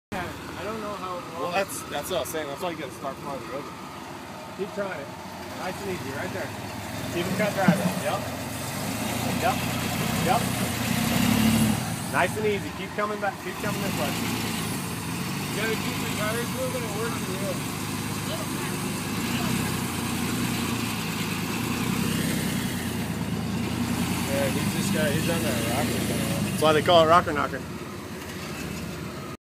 Eric Rocker knocker
Eric with his yota on Rocker Knocker